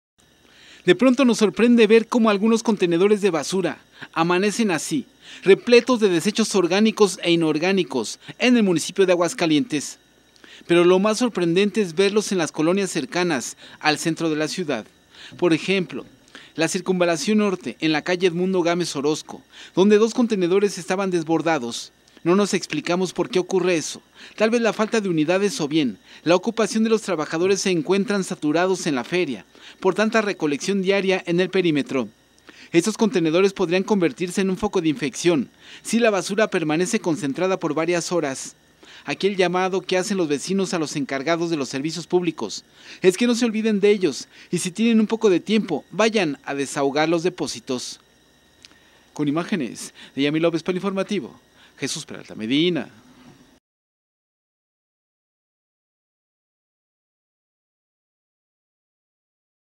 CONTENEDORES DE BASURA HASTA EL TOPE
Vecinos se quejan de que el servicio de recolección de basura se ve afectado, por la Feria de San Marcos.